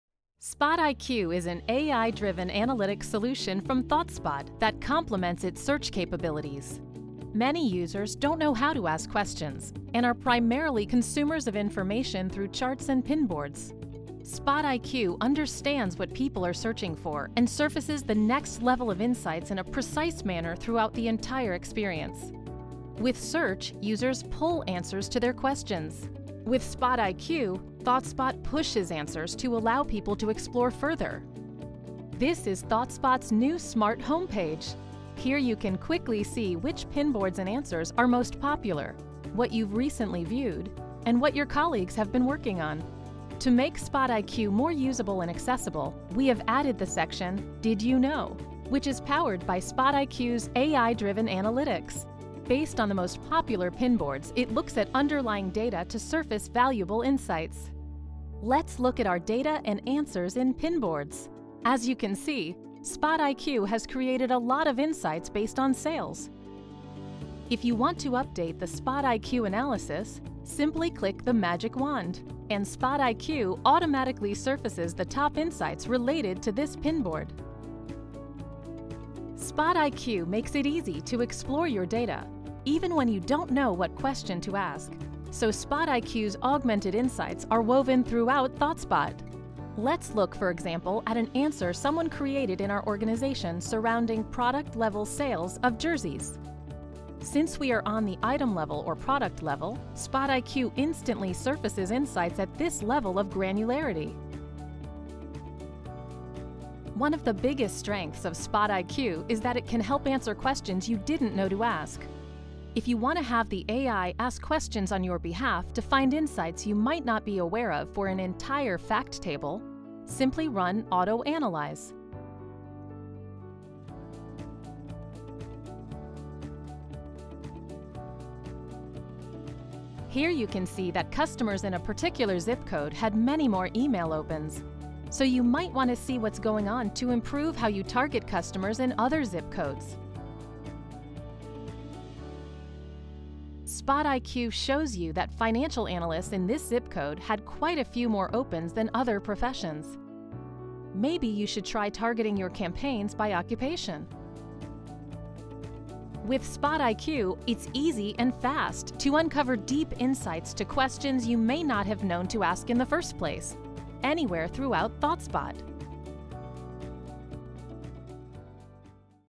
SpotIQ Pervasive Insights
Uncover deeper answers to known and unknown questions with “Did you know?” insights on your Smart Home Page and Instant Insights on answers and pinboards.